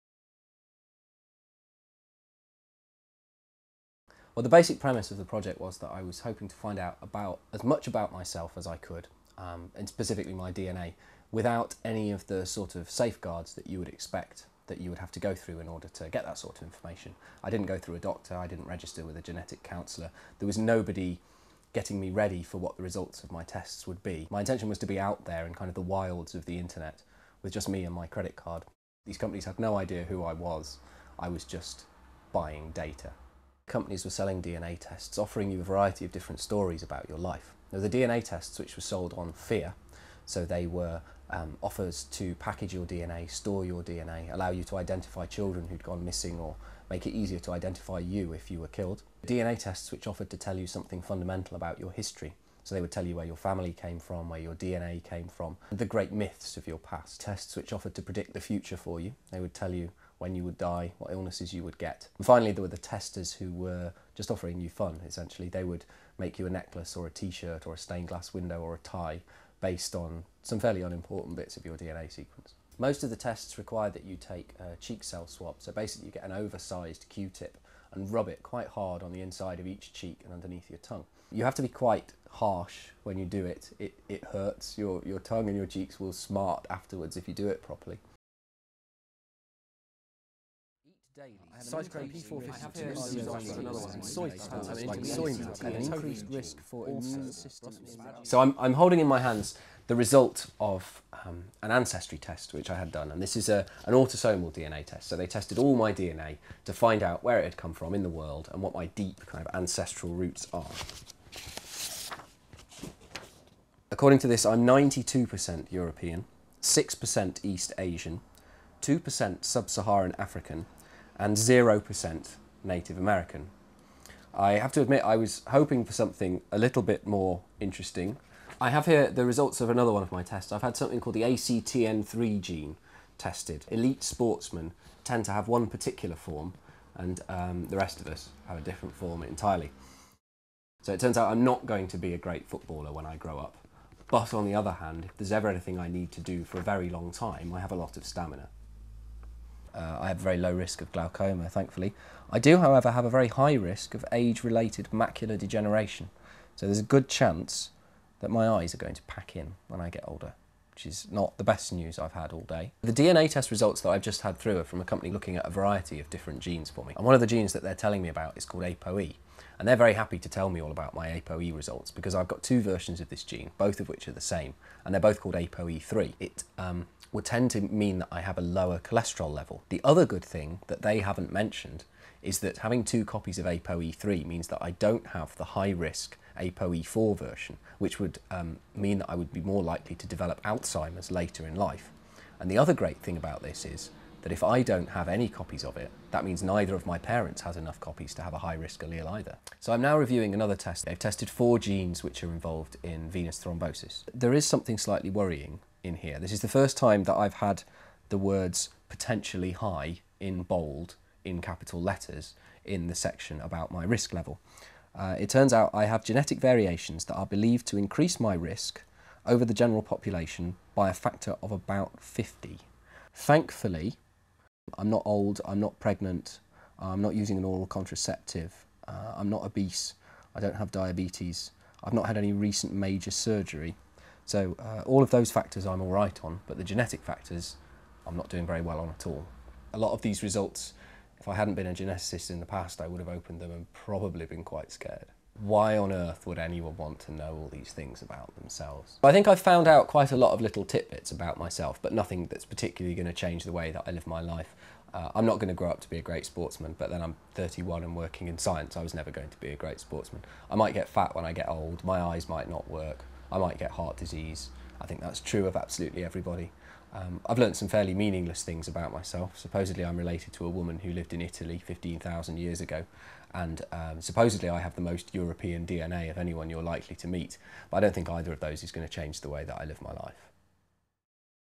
Over the counter testing
How curious are you about the information in your DNA? Have you ever thought of taking a genetic test you can buy on the internet? Public interest in DNA is increasing and people are more curious about the information contained within their genome. It is now possible to access a range of DNA tests -- for a price -- on the internet.

Yourgenome.org presents a short video of Dr Steve Cross who decided to analyse his DNA using tests available to UK citizens over the internet. You may find his thoughts and results interesting.